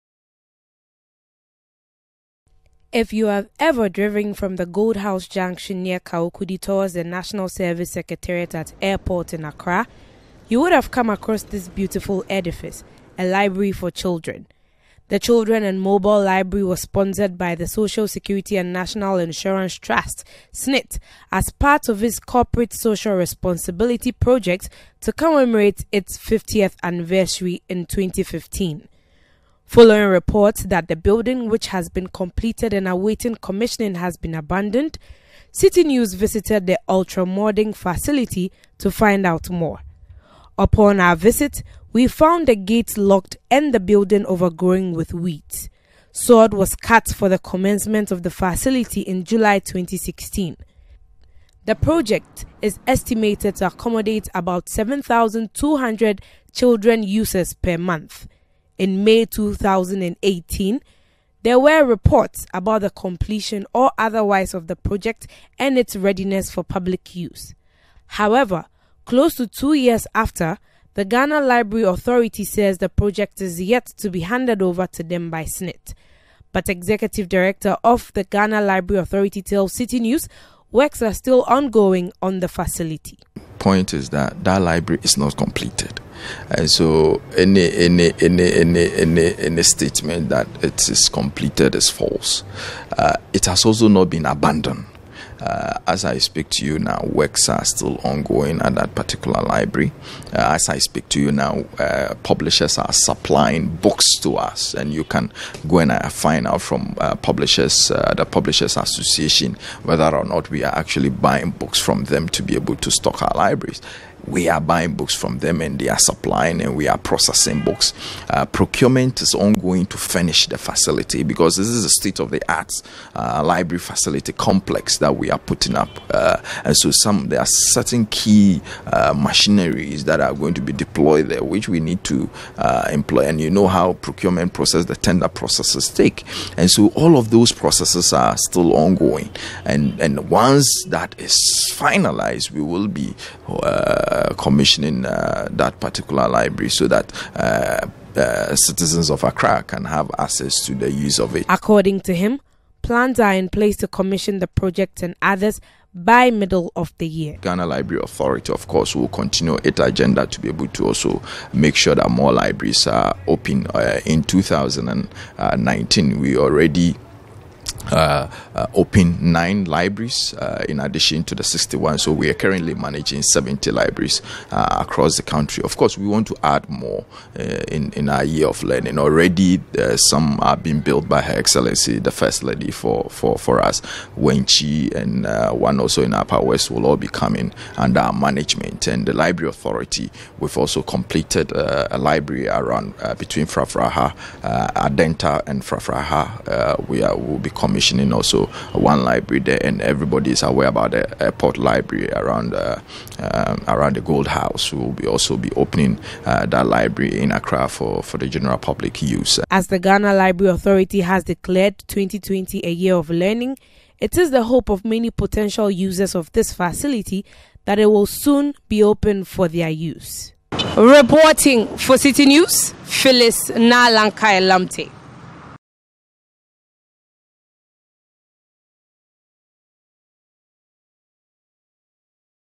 Ultra-Modern Kawukudi library: Ghana Library Authority refutes claims of facility been abandoned
The Ghana Library Authority has debunked claims that the newly built SSNIT-sponsored Children’s Library located at Kawokudi near the Gold House junction has been abandoned.

Executive Director of the Authority, Hayford Siaw tells Citi News that works on the library are still ongoing. Mr Siaw is however hopeful the Library will be open to the public by the middle of this year.